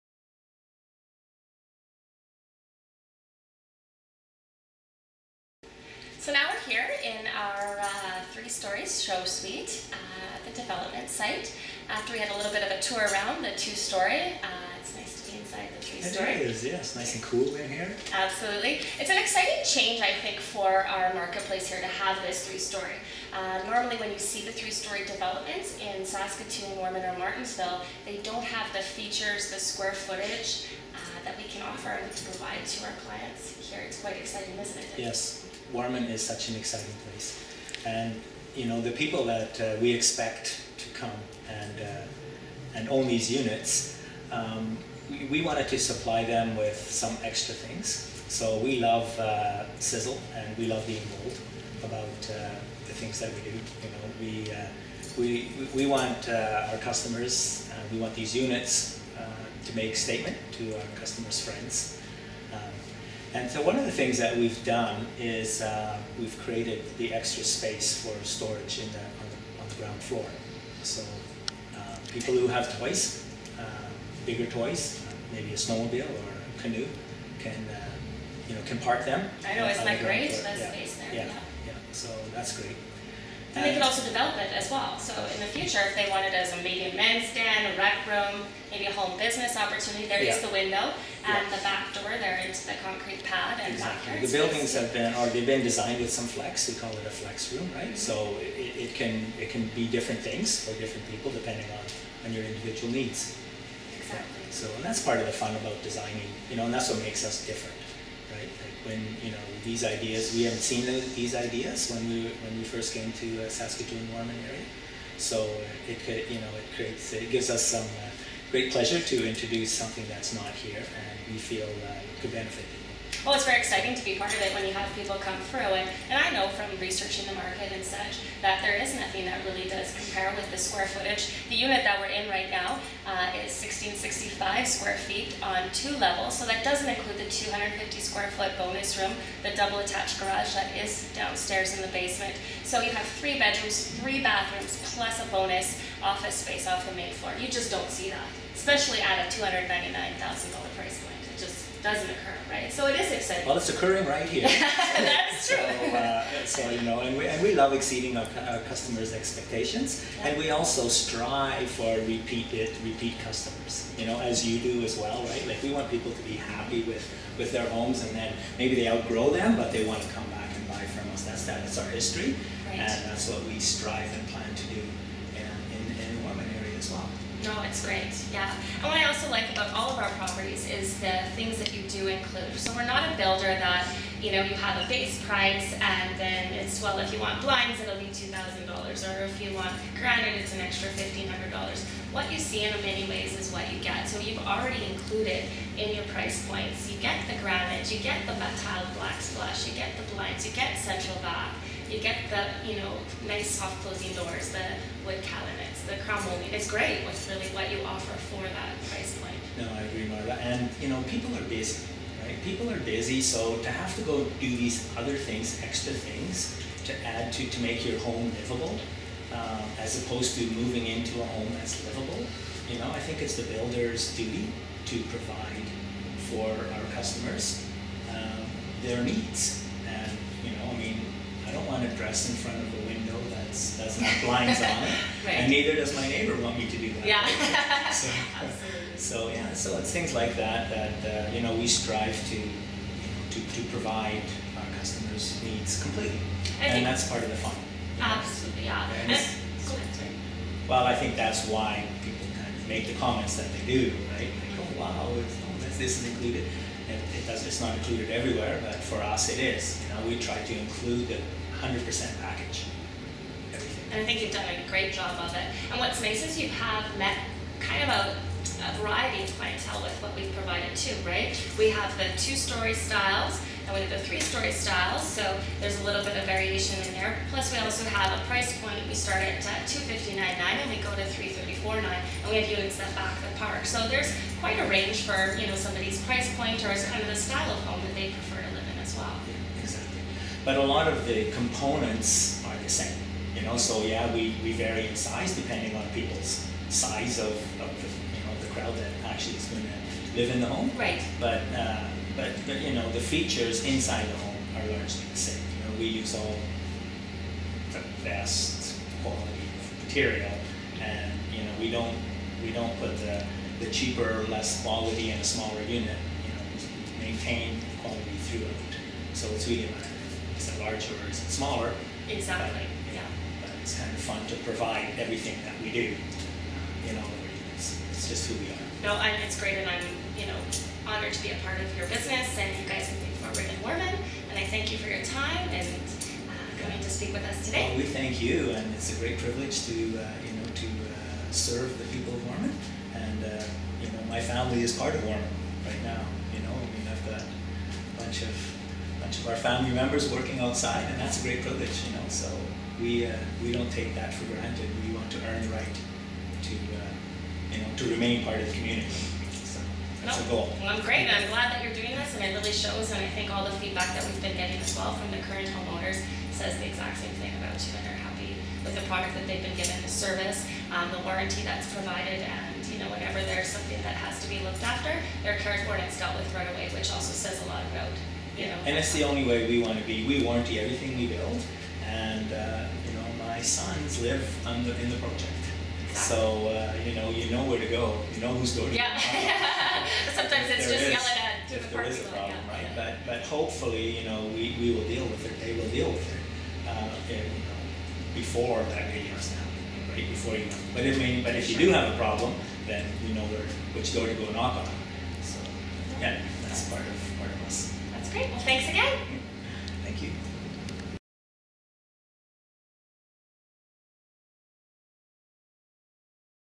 Trusted Saskatoon Real Estate Expert Marla & Dick the developer of Radius at the Legends Warman SK
Trusted Saskatoon Real Estate expert Marla Janzen on Century 21 Fusion chats with Dick the owner of Westbow Construction about the stunning condo project in Warman SK.